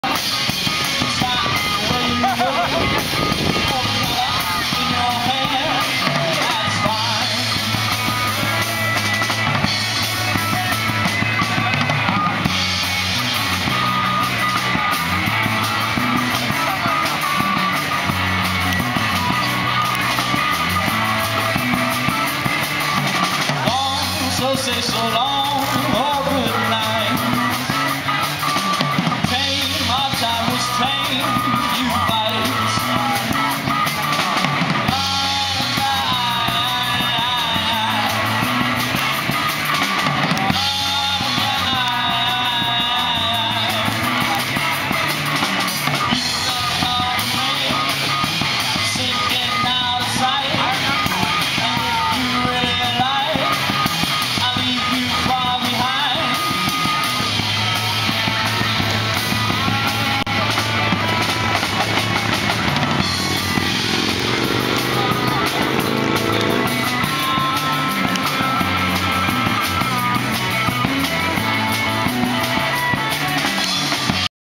2011.06.25 Mount Joy playing live at Clark Park Summer Festival 2011
My new fav band Mount Joy playing at this years Clark Park Summer Solstice Festival in West Philly, June 25th, 2011